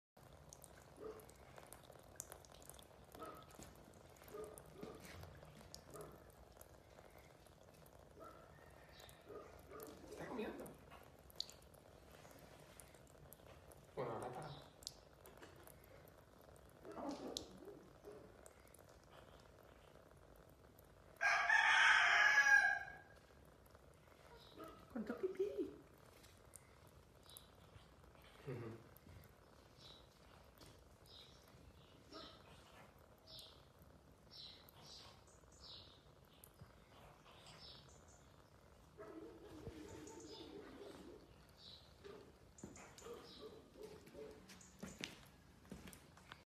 Noel, un chivo con meningitis
Hemos sacado a Noel a que le dé el ☀️